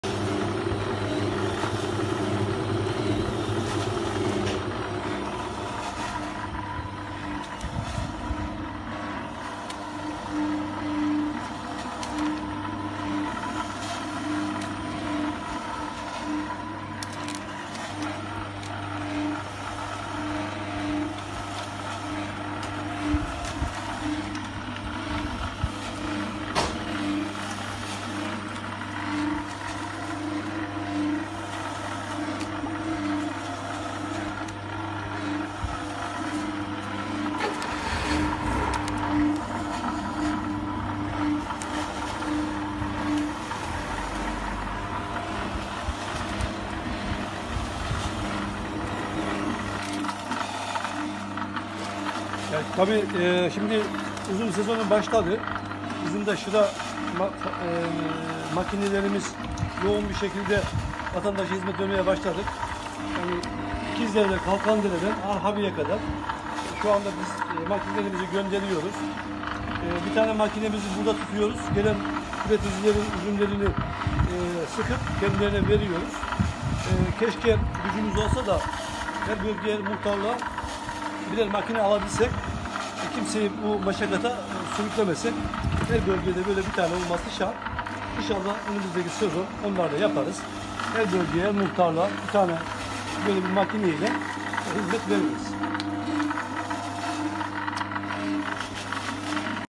Rize’de Kokulu Üzümler Toplanıyor Pekmez Mesaisi Başladı
Çay Üreticileri Dayanışma Derneği (ÇAYÜDAD) Başkanı Mustafa Mavi, vatandaşların kaliteli ürüne en az zayiatla ve en kısa zamanda ulaşmasını sağlamak üzere üzüm sıkma işlemine başladıklarını söyledi.
Çay Üreticileri Dayanışma Derneği (ÇAYÜDAD), Rize'ye özgü bir tatlı olan pepeçura ve pekmez yapımında kullanılan kokulu üzüm suyunu sıkma noktasında üreticilere büyük kolaylık sağlıyor. Asmalardan üzümleri toplayan vatandaşlar pekmez yapımı için kolları sıvarken ÇAYÜDAD da vatandaşların topladıkları üzümlerin suyunu makine ile ücretsiz bir şekilde sıkarak şıraya dönüştürüyor. Bu sayede de vatandaşların pekmez kazanları üzüm suyuyla dolup taşıyor.

ÇAYÜDAD Başkanı Mustafa Mavi, yaptığı açıklamada, 2022 yılı üzüm şırasına başladıklarını ifade ederek, İkizdere’den, Kalkandere’den Arhavi’ye kadar hizmet verdiklerini kaydetti.

Üzüm sıkma işlemine yoğun bir talep olduğunu belirten ÇAYÜDAD Başkanı Mavi, vatandaşların kendilerinden haber almadan üzüm toplamamalarını istedi. Mavi, hedeflerinin tüm muhtarlıklara birer adet üzüm sıkma makinesi almak olduğunu belirtti.

Mavi, açıklamasında şunları kaydetti: “Birçok yetiştiricinin geleneksel yöntemlerle suyunu çıkardığı kokulu siyah üzümde üreticinin işini kolaylaştırmak için almış olduğumuz ve önceki yıllarda da üzüm sıkma işleminde bulunduğumuz üzüm sıkma makineleriyle 2022 yılı üzüm şırasına başladık. Şu an yoğun bir talep var. Şıra makinelerimiz yoğun bir şekilde vatandaşlara hizmet vermeye başladı. İkizdere’den, Kalkandere’den Arhavi’ye kadar makinelerimiz hizmet veriyor. İki makinemizi ilçelere gönderirken bir tane makinemizi burada tutuyoruz. Gelen üreticilerimizin üzümlerini sıkıp kendilerine veriyoruz. Geçtiğimiz yıl 200-250 ton civarında üzüm sıkma işlemi gerçekleştirmiştik. Bu sene ise 500 tona yakın üzüm sıkma işlemi gerçekleştireceğimizi düşünüyoruz. Vatandaşlarımız acele etmesin, bizden haber almadan üzüm toplamasın. Derneğimizin Engindere Mahallesinde bulunan ofisinde inşallah herkesin üzümünü sıkıp pekmez yapmalarını kolaylaştıracağız. Keşke gücümüz olsa da her bölgeye her muhtarlığa bir makine alabilsek de kimseyi bu meşakkata sürüklemesek. Her bölgeye bir makine şart. İnşallah önümüzdeki sezon onu da yapar, her bölgeye her muhtarlığa makine alabiliriz” diye konuştu.

ÇAYÜDAD tarafından hayata geçirilen üzüm sıkma projesi geçtiğimiz yıl Ziraat Odasına önerilmiş, Ziraat Odası ise projeyi onaylamamıştı. Projenin onaylanmaması üzerine ÇAYÜDAD, projeyi AK Parti Genel Başkan Yardımcısı ve Rize Milletvekili Hayati Yazıcı’ya sunmuş, Yazıcı da projeye destek vererek 3 adet üzüm sıkma makinesini ÇAYÜDAD’a hediye etmişti.